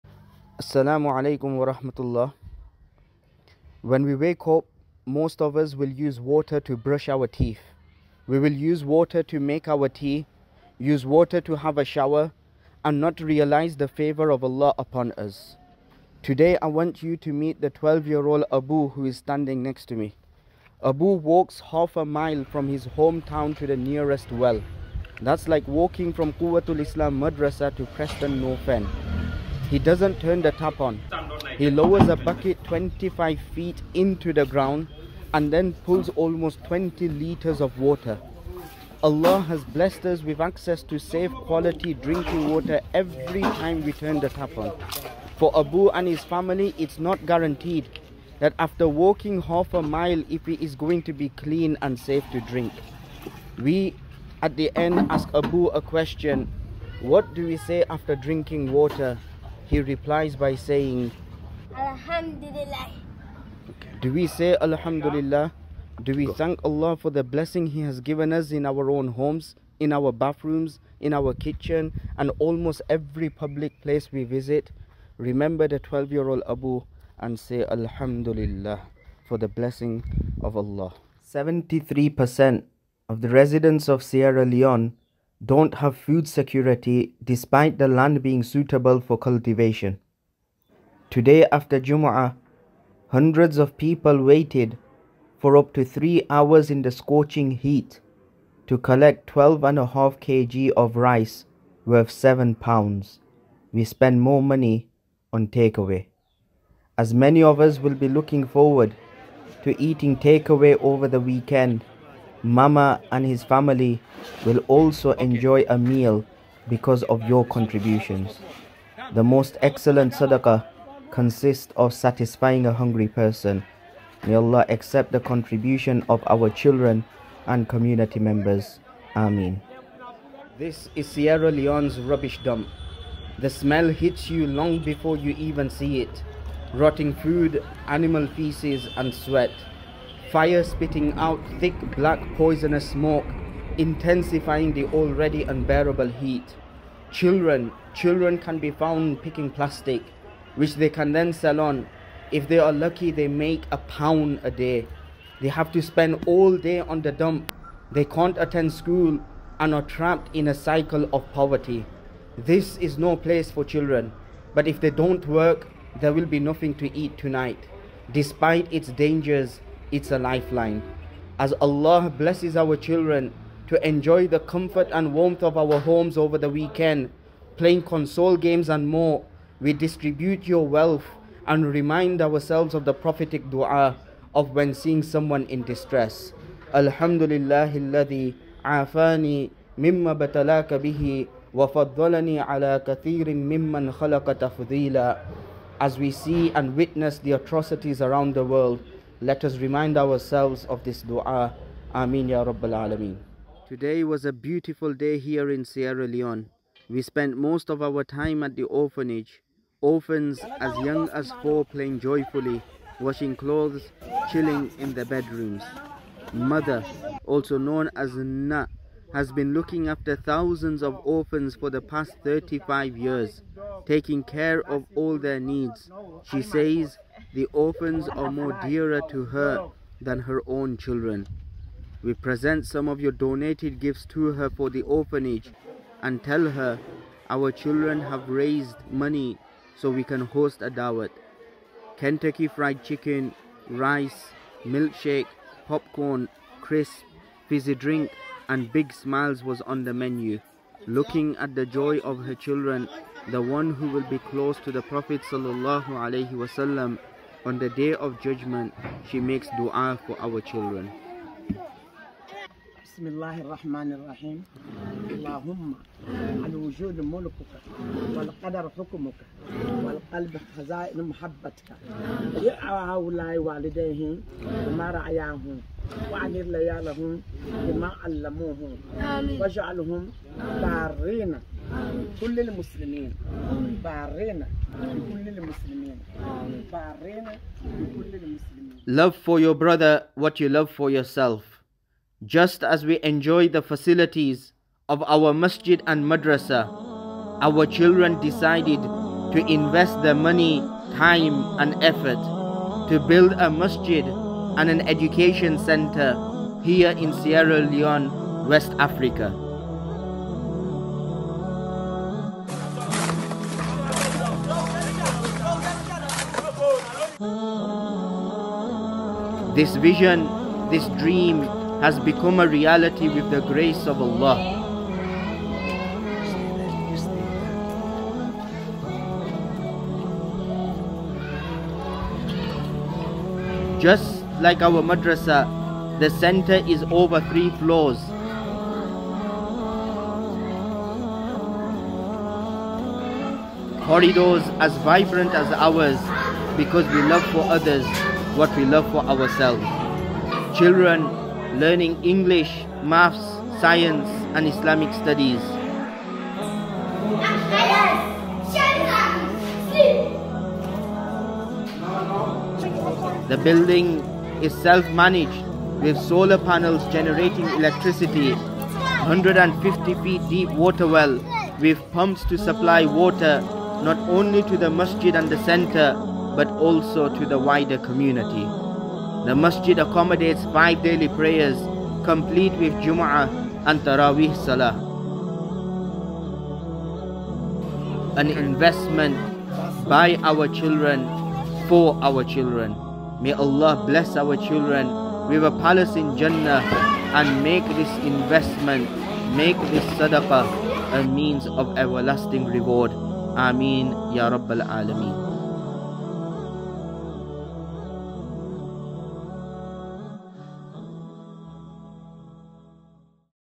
Sierra Leone 2023
00:00 💧 A blessing of Allah
1:21 Your contribution making a difference
2:10 Action Alert, learn & pray the dua
3:23 A blessed dawat by our pupils in Y7+
4:48 Quwwatul Islam Centre